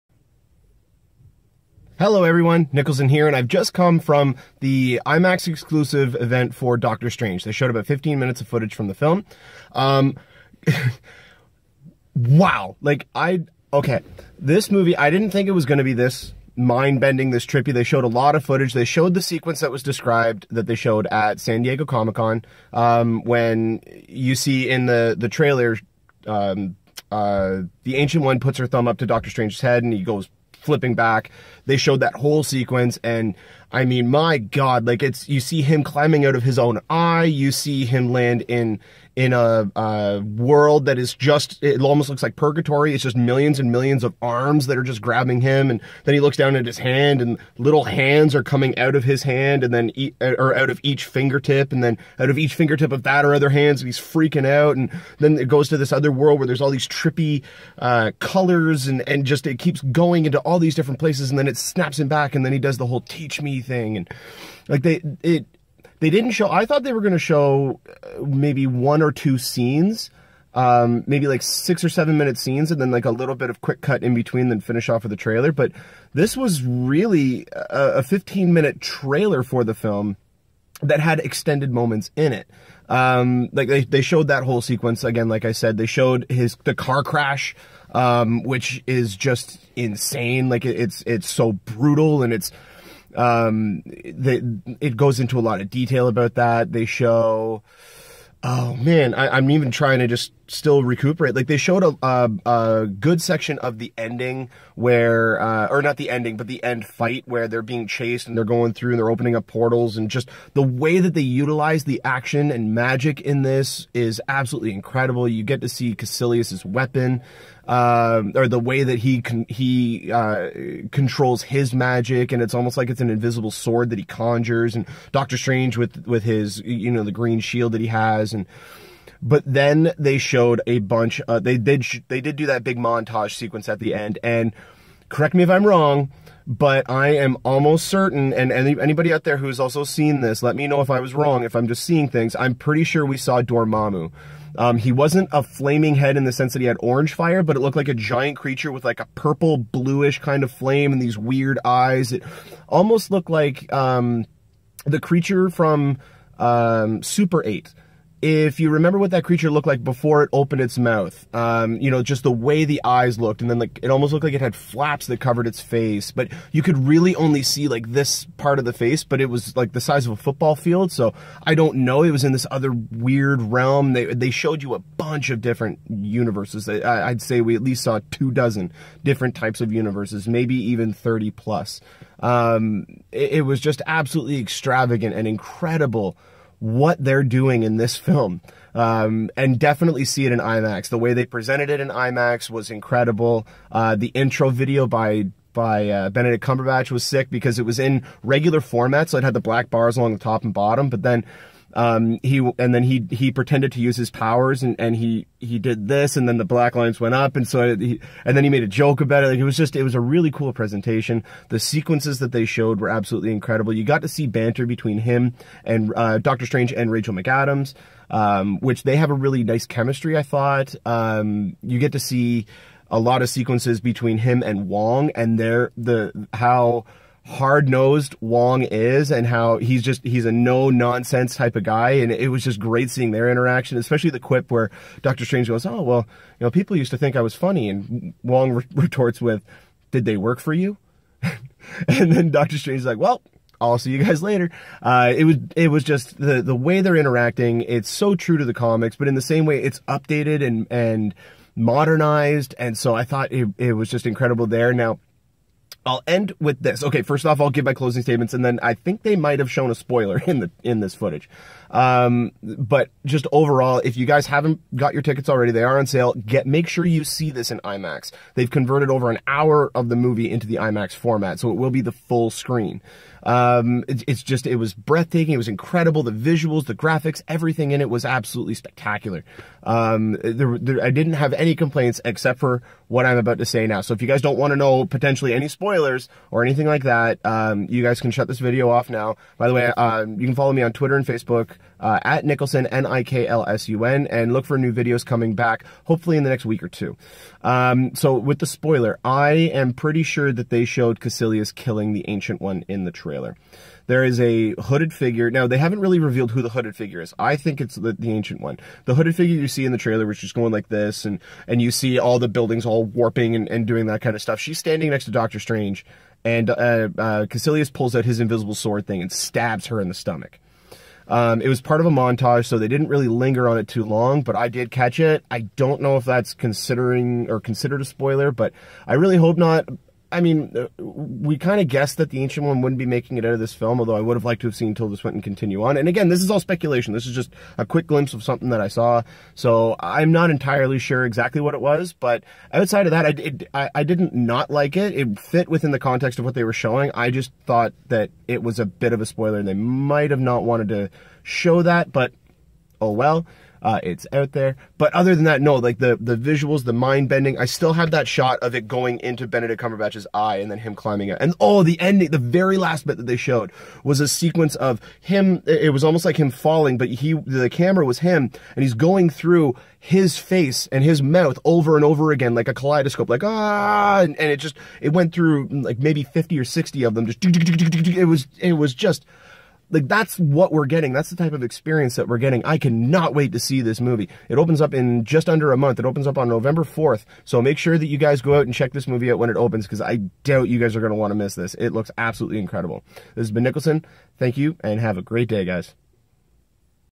DOCTOR STRANGE IMAX Event Review! - Movie News
Marvel Studios debuted 15 minutes of footage in select IMAX screens around the US and Canada. I attended this exclusive event, watch the video for my